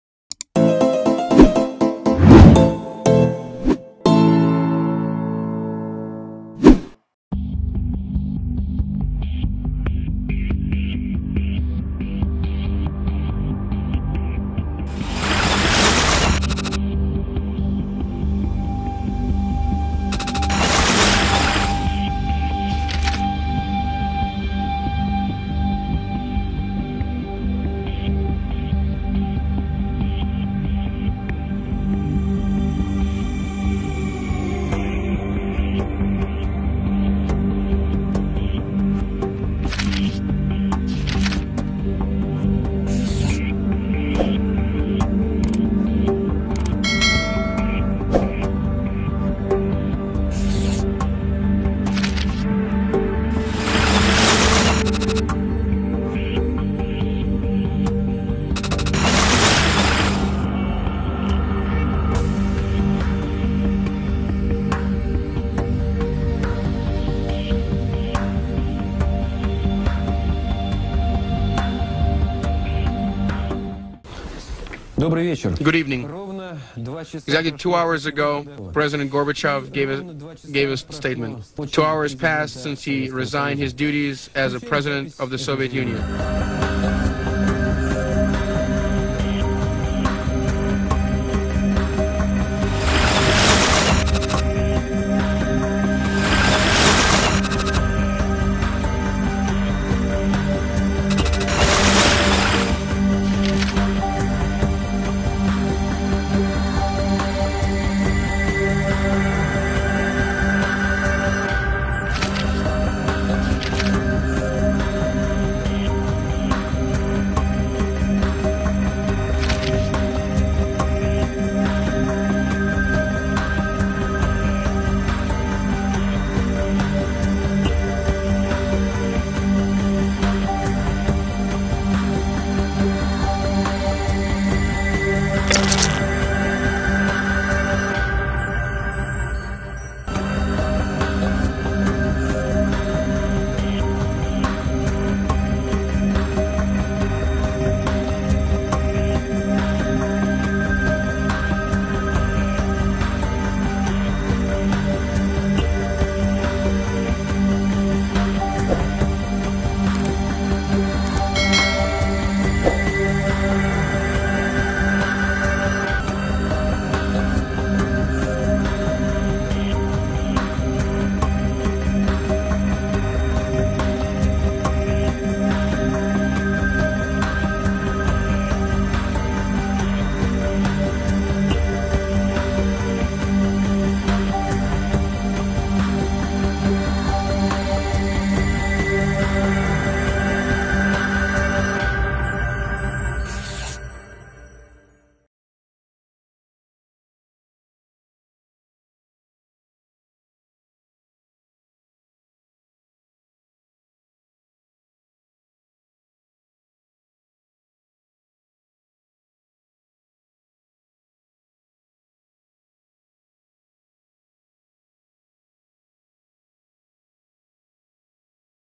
تاريخ لا ينسي | 03 | اليوم الأخير في الإتحاد السوفيتي!!
تاريخ_لا_يُنسي (3) : ماذا حدث في اليوم الأخير في الاتحاد السوفيتي؟!
إنهيار الإتحاد السوفيتي (26 ديسمبر 1991)...
♦ لو عجبك المحتوي ياريت تدعمنا بإشتراك في القناة ولايك لصفحة الفيسبوك ♥